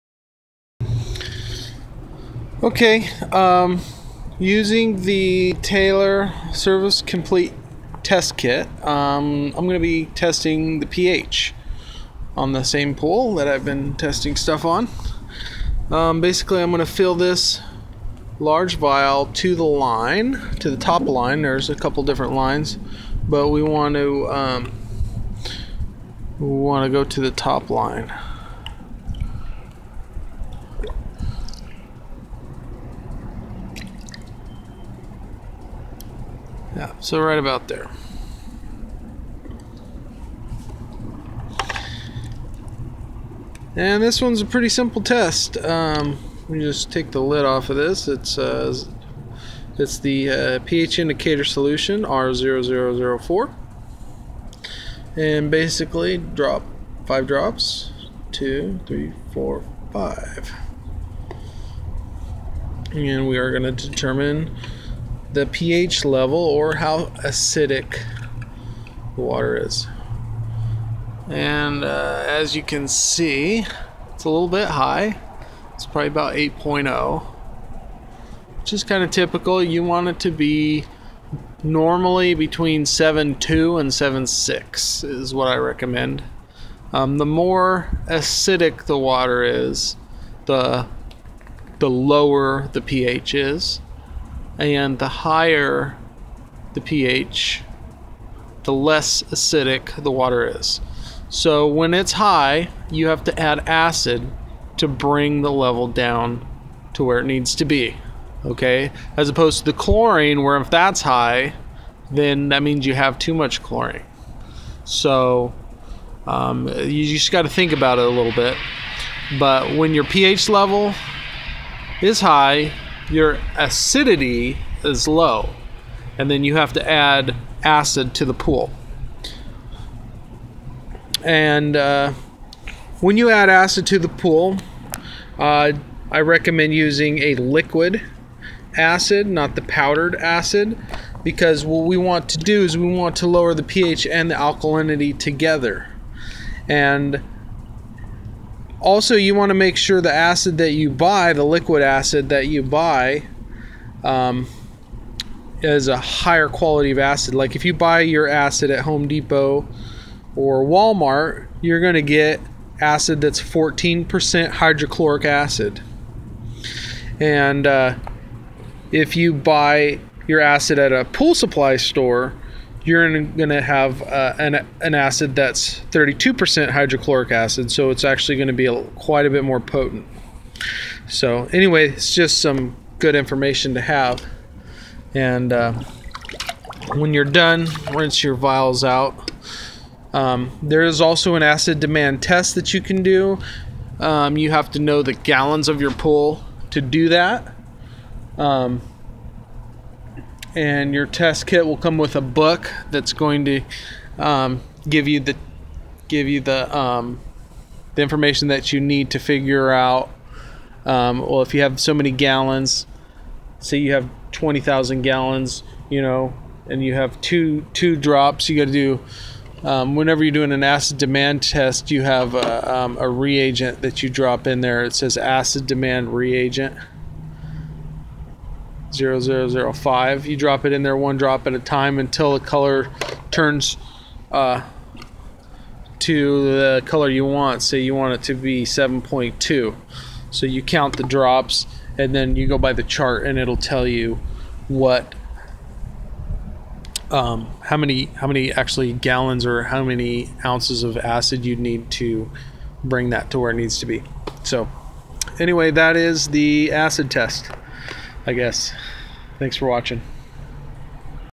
Testing The PH On Your Pool
How To Test The PH On Your Swimming Pool Using A Taylor Test Kit